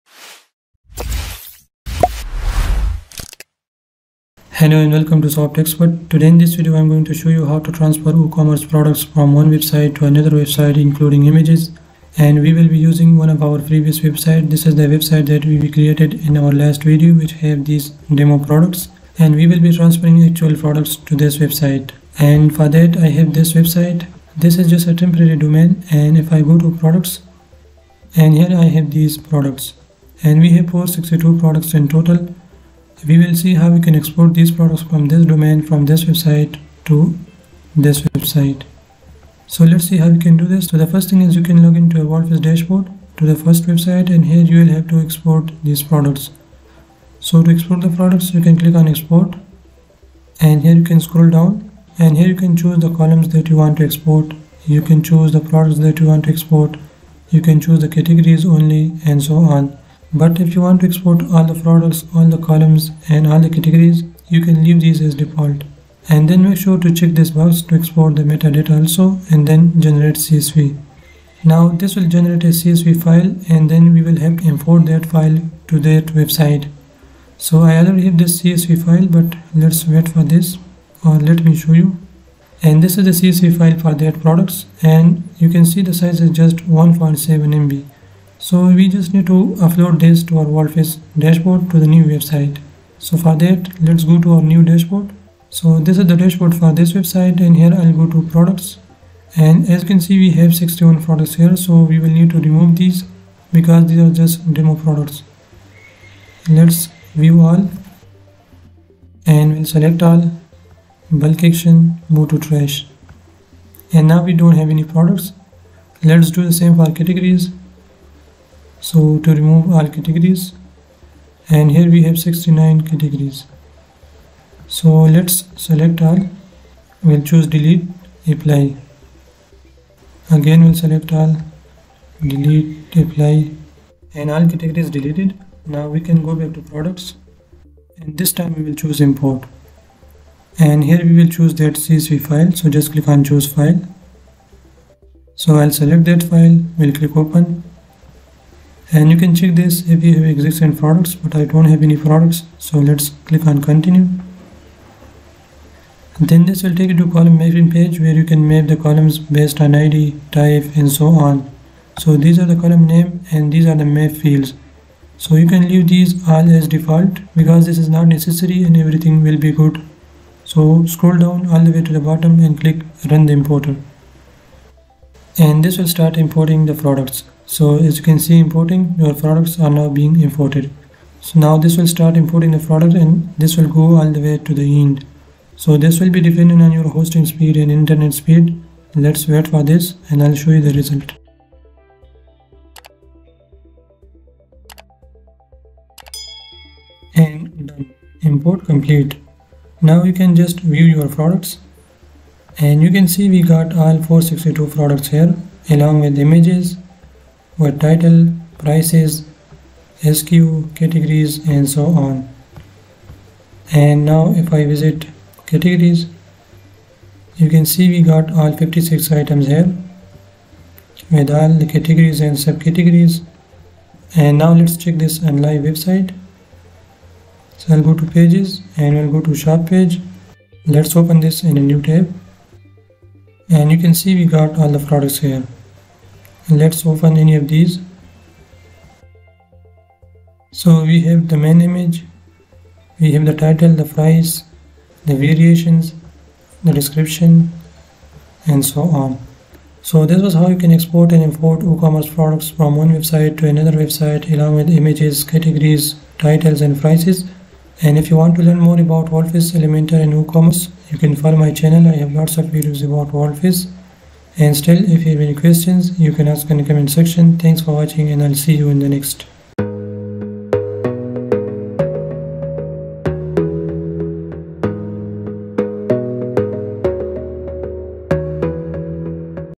How to Export and import WooCommerce Products with images
Let’s see How to Export and import WooCommerce Products with images.